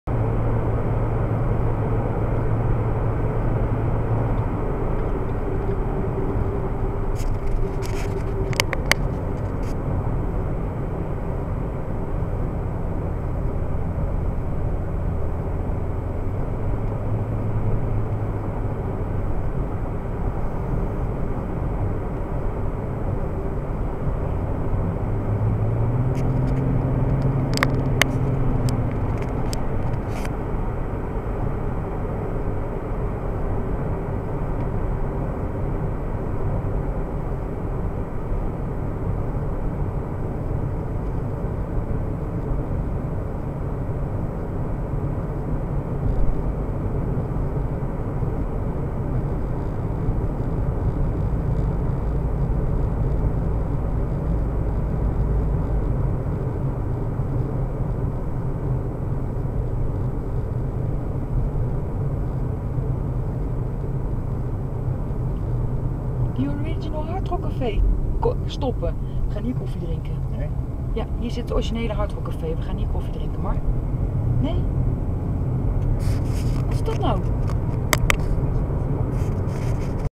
USA Coast to coast: Colorado, nabij Empire
USA Coast to coast: wij reden in 3 weken van Washington DC aan de oostkust door 16 staten en in 9.920 km naar Los Angeles aan de westkust van de US of A.